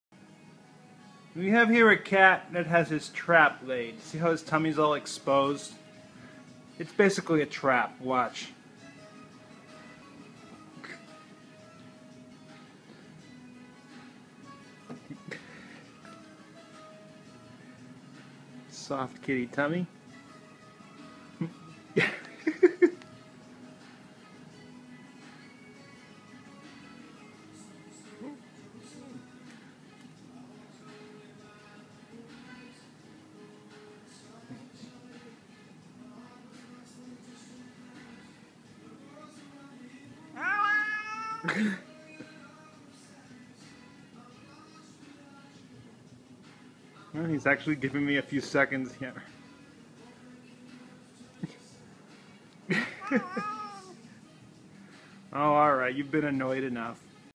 The kitty trap
Kitty tummies, soft and inviting as they may seem, are a trap! :)